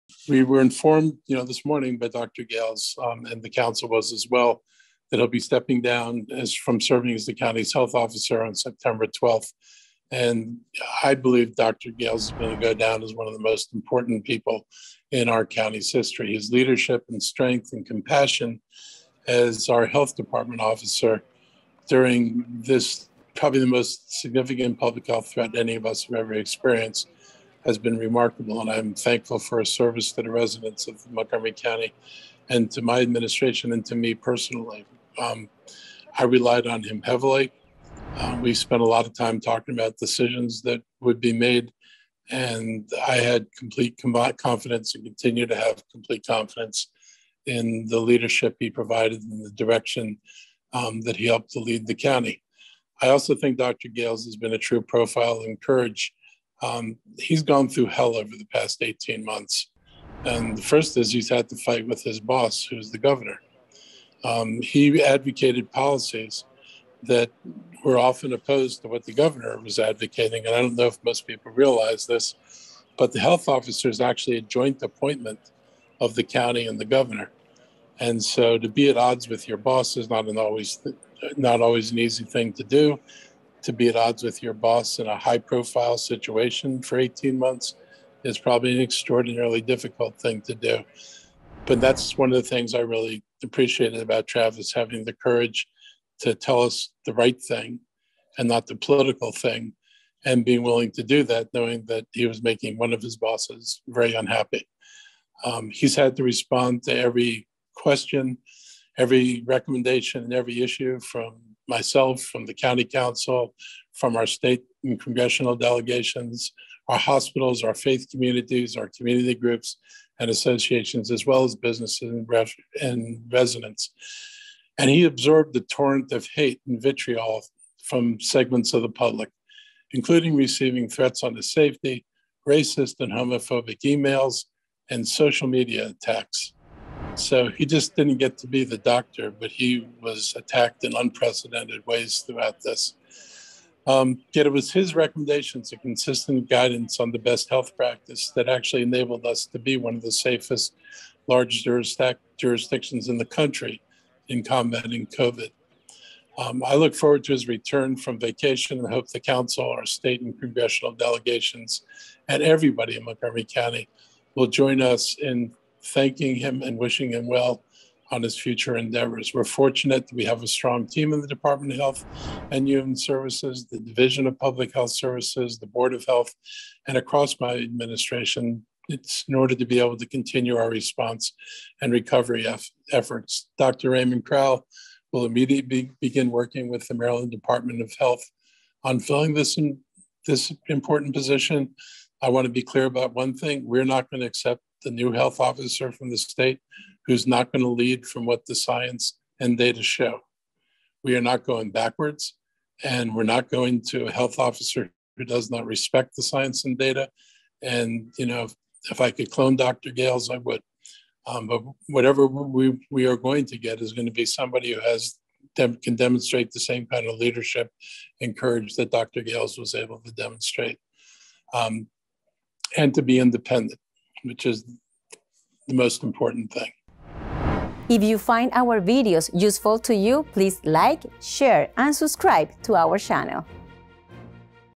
Elrich on County Health Officer Resignation: 'If I Could Clone Dr. Gayles, I Would'
Local leaders have been informed that Montgomery County Health Officer Dr. Travis Gayles will be resigning, and his last day is on Sept. 12.

"I believe Dr. Gayles is going to go down as one of the most important people in our county's history. It was his recommendations and consistent guidance on the best health practice that actually enabled us to be one of the safest, largest jurisdictions in the country in combating COVID," said Montgomery County Executive Marc Elrich.

During his weekly virtual media briefing on Wednesday, Elrich mentioned how he felt about working with Dr. Gayles.